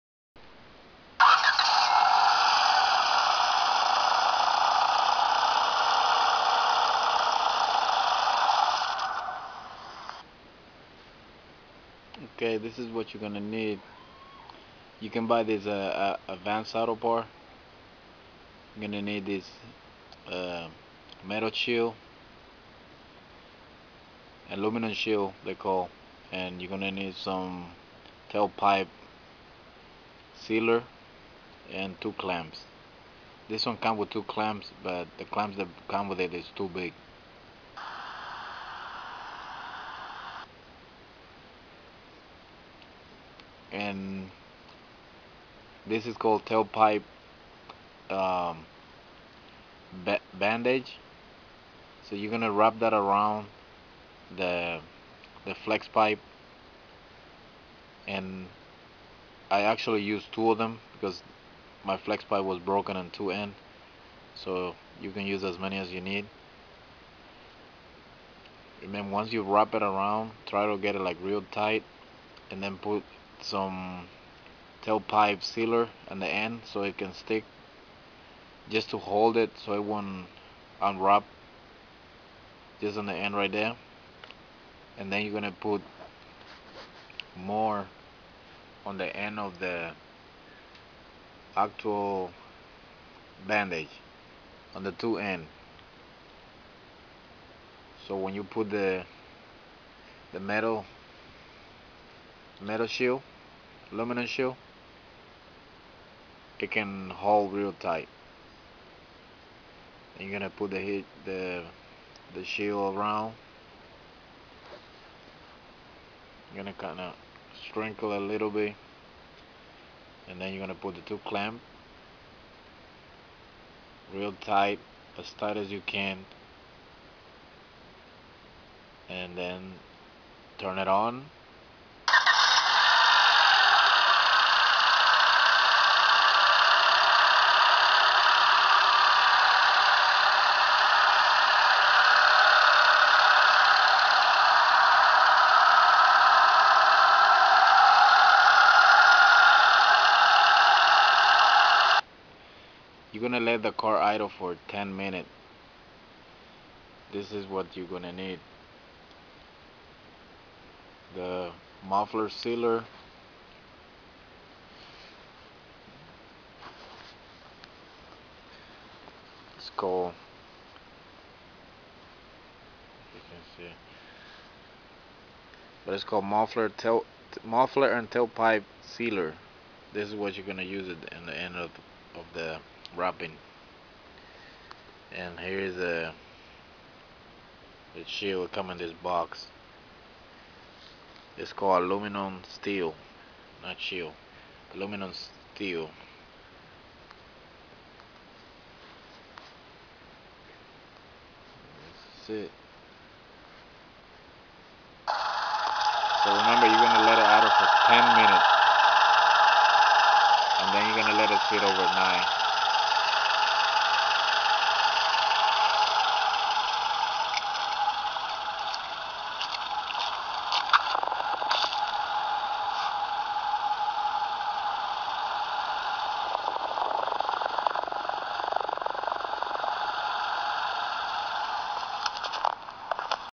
How to fix flex pipe on a car
This is the way you fix your flex pipe at home go to the auto parts and get Aluminum steel,Muffler & tailpipe Sealer, and two  clamps 2-3/4" and  Tailpipe Bandage